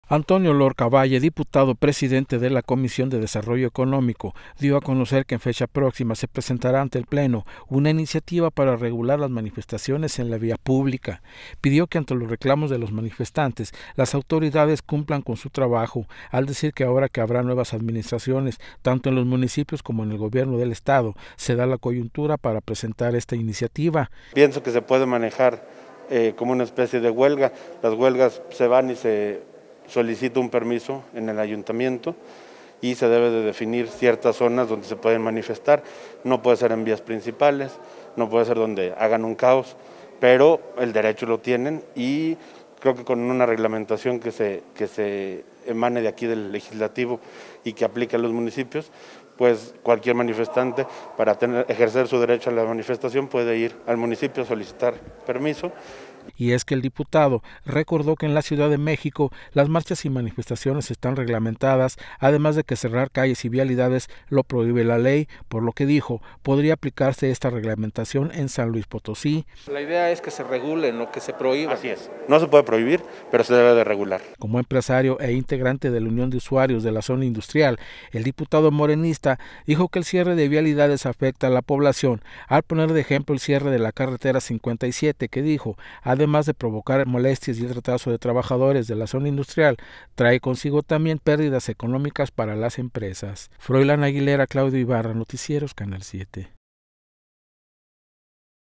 Lorca Valle presentará propuesta para reglamentar las marchas y manifestaciones en la vía pública.
• Se busca evitar conflictos y pérdidas económicas.

SLP.- Antonio Lorca Valle, diputado presidente de la Comisión de Desarrollo Económico, dio a conocer que en fecha próxima se presentará ante el Pleno, una iniciativa para regular las manifestaciones en la vía pública.

Pidió que ante los reclamos de los manifestantes las autoridades cumplan con su trabajo al decir que ahora que habrá nuevas administraciones, tanto en los municipios como en el gobierno del estado, se da la coyuntura para presentar esta iniciativa.

Y es que el diputado recordó que en la Ciudad de México las marchas y manifestaciones están reglamentadas además de que cerrar calles y vialidades lo prohíbe la ley, por lo que dijo, podría aplicarse esta reglamentación en San Luis Potosí.

Como empresario e integrante de la Unión de Usuarios de la Zona Industrial el diputado morenista dijo que el cierre de vialidades afecta a la población, al poner de ejemolo el cierre de la carretera 57 que dijo, además de provocar el molestias y el retraso de trabajadores de la Zona Industrial trae consigo también pérdidas económicas para las empresas.